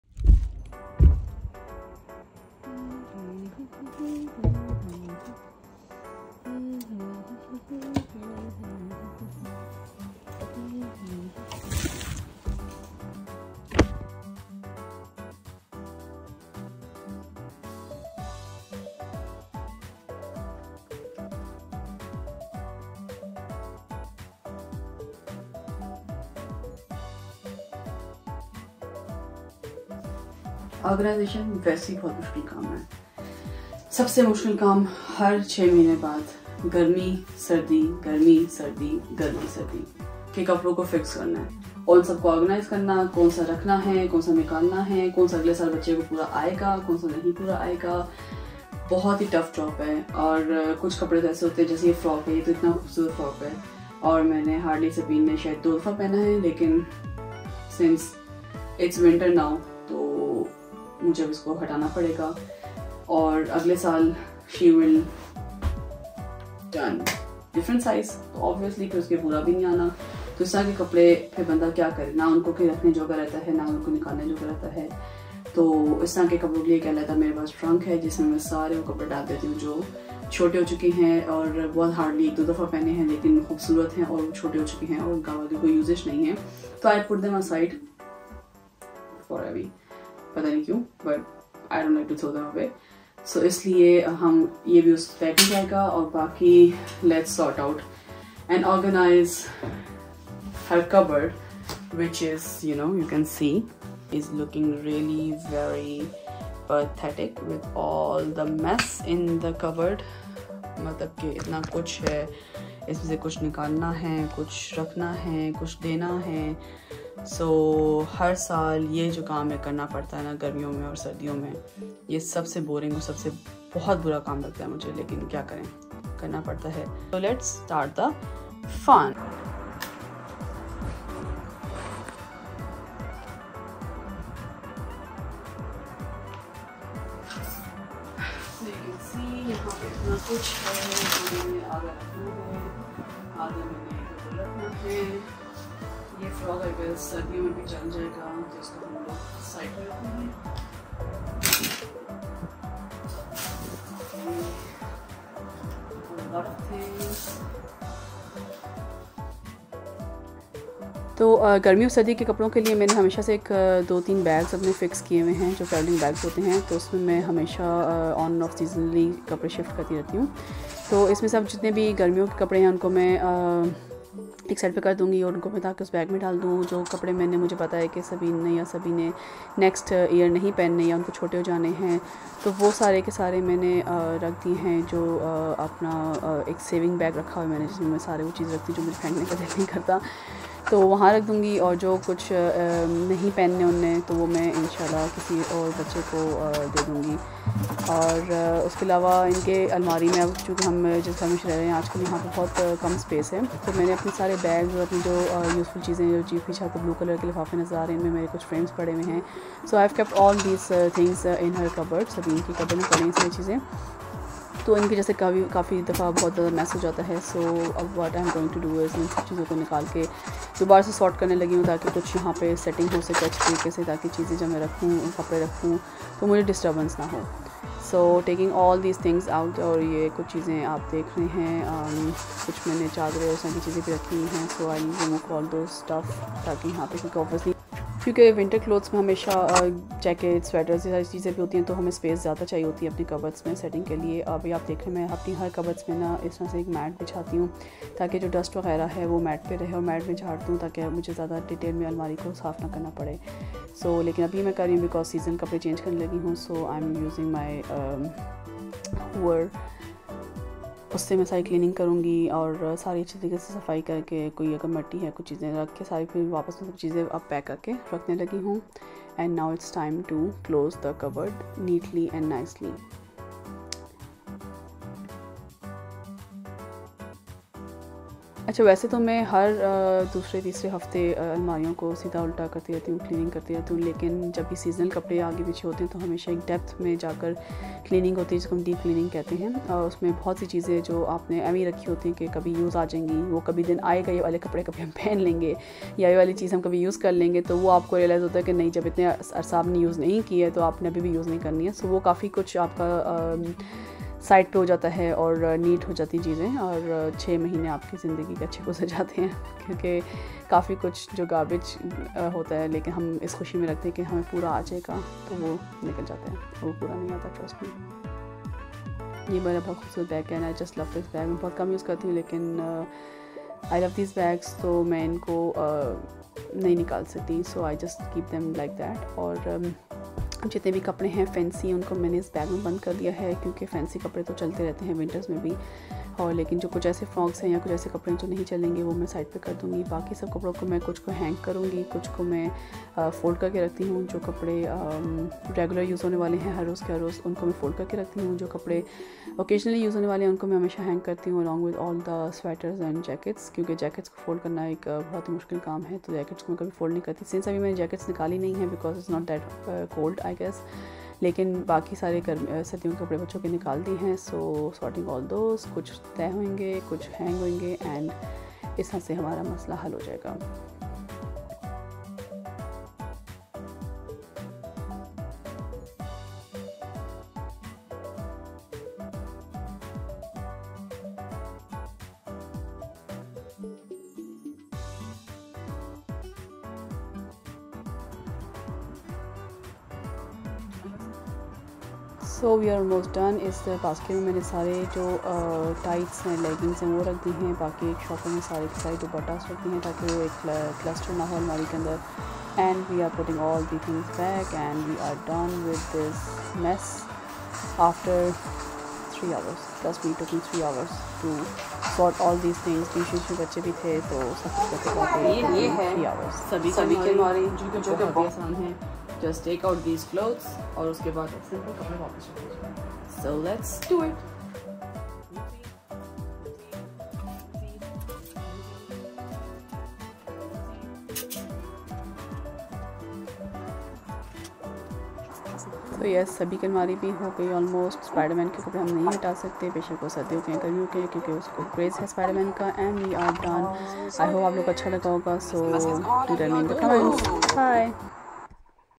Decluttering Seasonal Clothes: Organize Your Wardrobe with Me!
In this video, join me as we tackle the task of decluttering seasonal clothes. A well-organized wardrobe not only saves you time but also helps you appreciate the clothes you truly love.